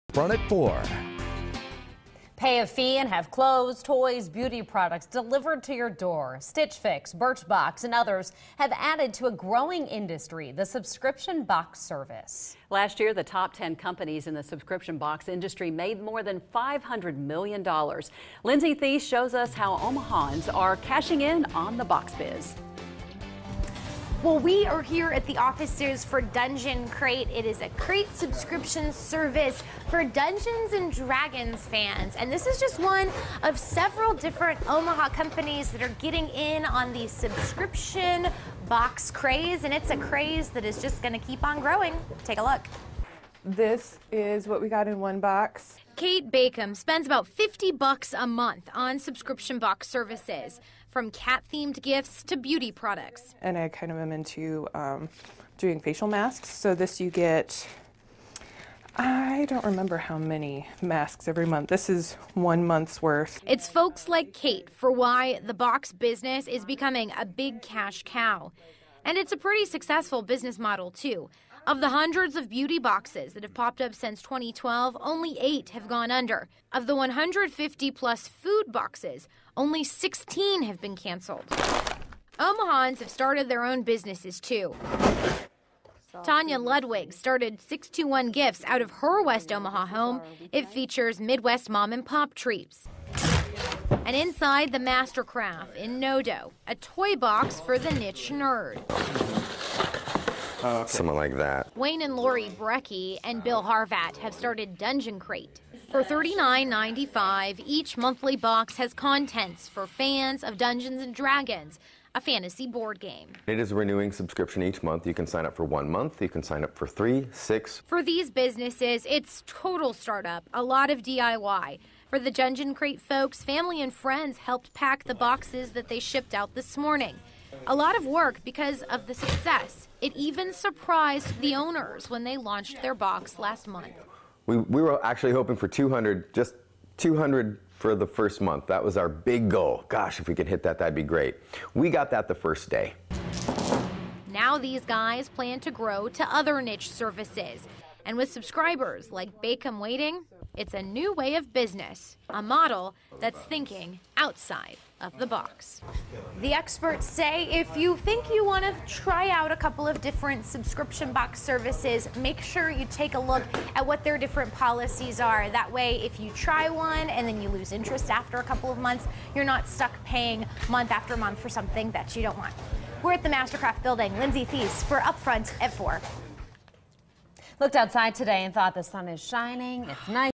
Omaha cashes in on subscription box services
Pay a fee and have clothes, toys, beauty products delivered to your door.  Stitchfix, Birchbox and others have added to a growing industry -  the subscription box service.